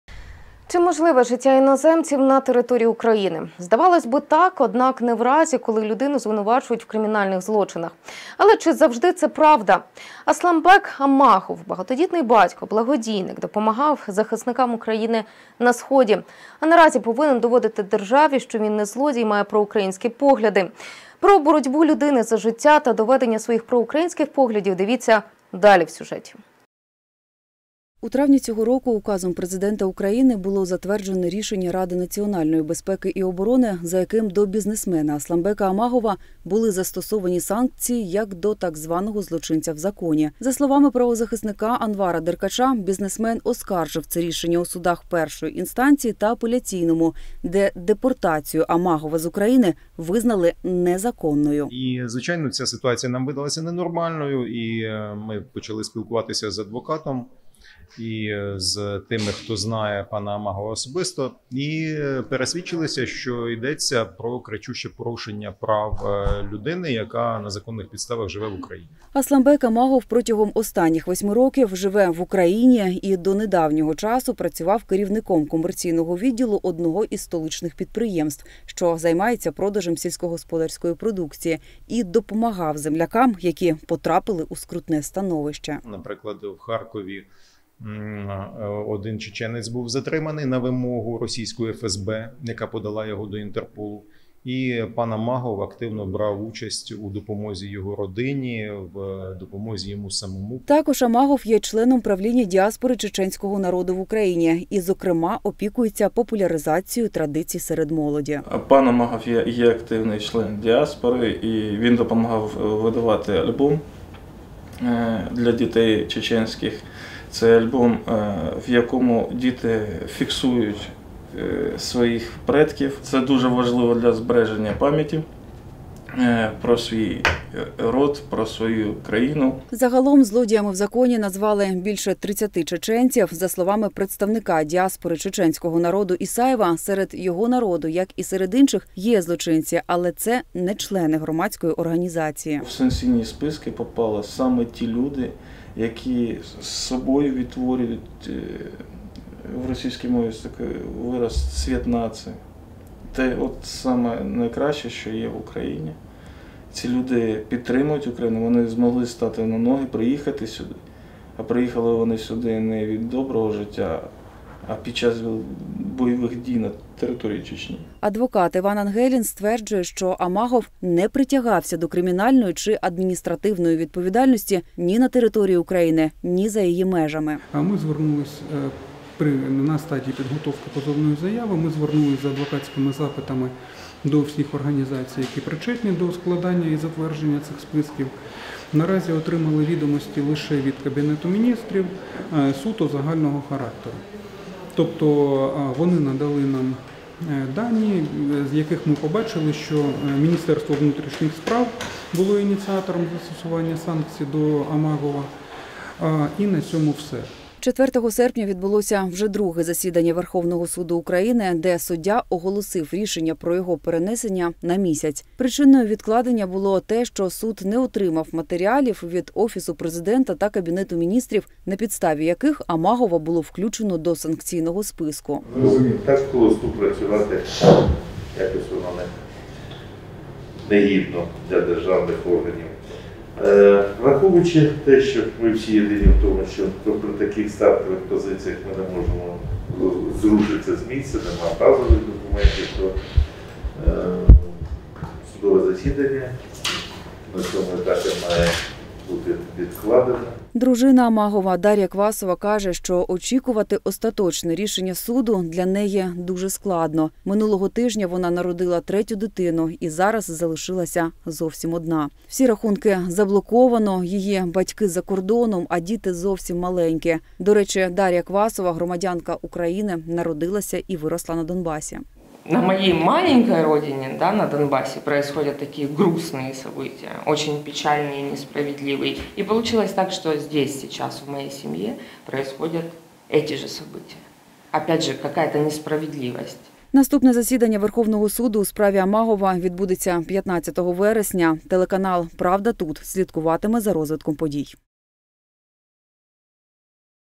Боротьба Амагова проти репресій в Україні
У Верховному Суді України відбулося друге засідання у справі бізнесмена Асламбека Амагова. Чоловік потрапив до так званого списку “злочинці в законі” і тепер йому загрожує депортація.  На захист Амагова стають, як правозахисники, так і громадські організації.

У травні цього року указом Президента України було затверджене рішення Ради національної безпеки і оборони, за яким до бізнесмена Асламбека Амагова були застосовані санкції, як до так званого "злочинця в законі".

Журналісти: Ася Хазова, Роман Ніколайчук